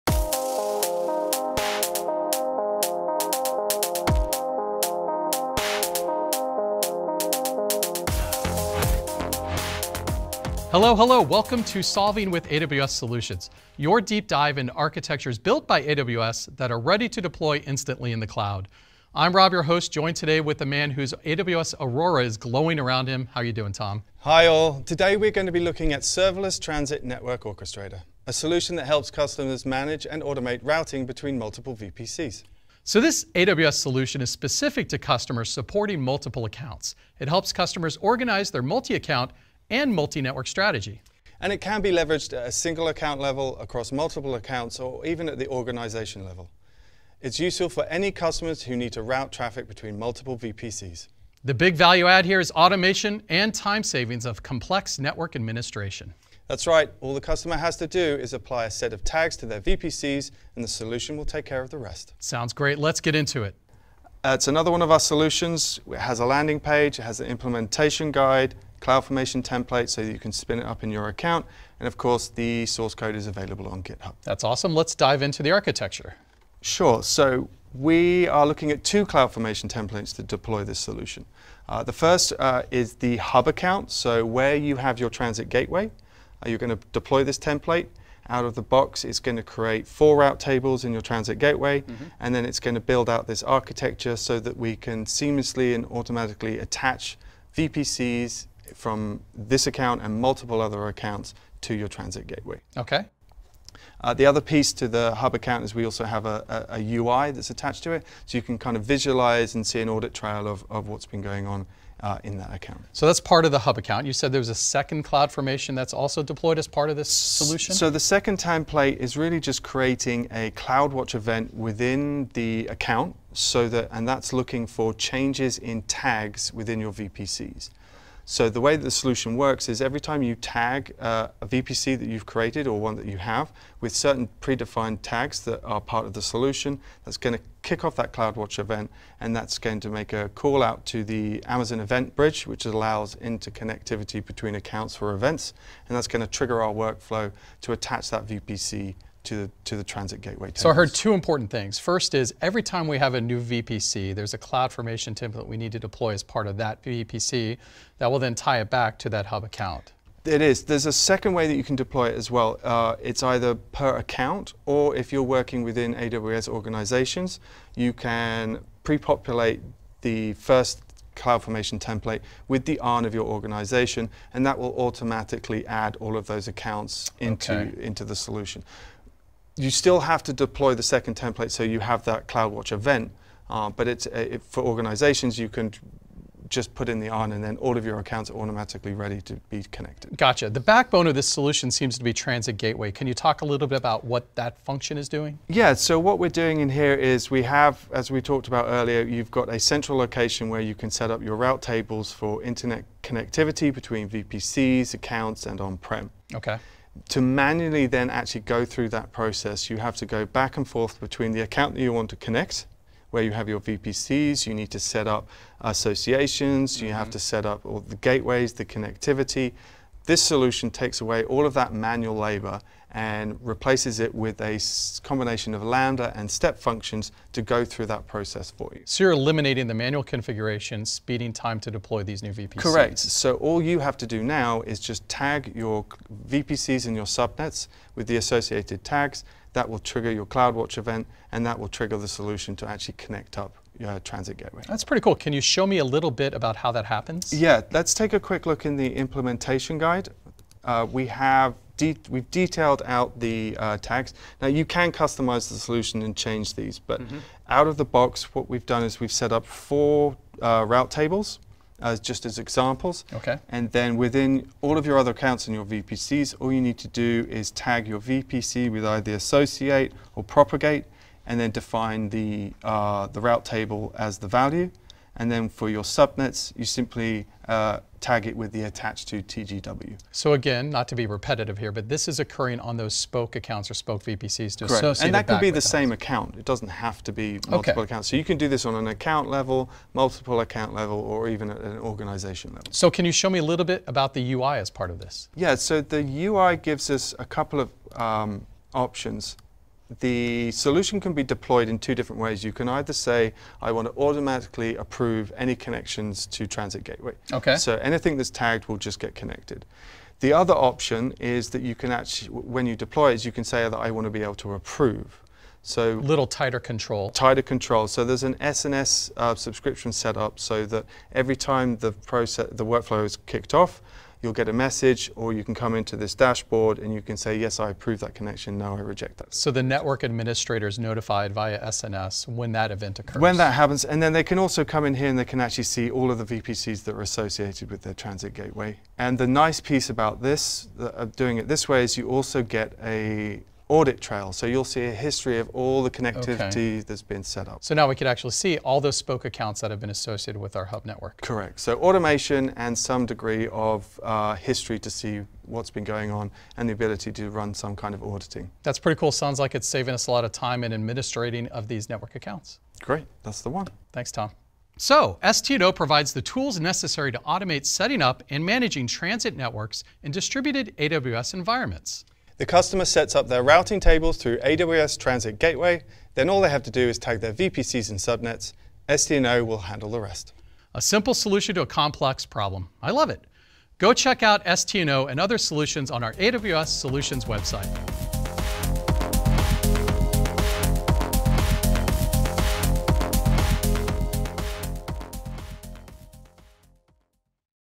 Solving with AWS Solutions: Network Orchestration for AWS Transit Gateway | Amazon Web Services
The Network Orchestration for AWS Transit Gateway solution adds automation to AWS Transit Gateway. This AWS Solution provides the tools necessary to automate the process of setting up and managing transit networks in distributed AWS environments.

AWS Solutions Library